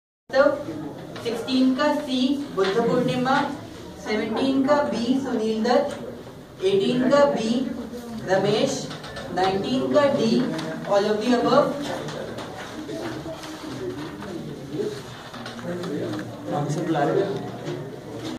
The Survivor Copy Checking Round 4
All team members checking copy while Saurabh Bhatnagar is dictating answers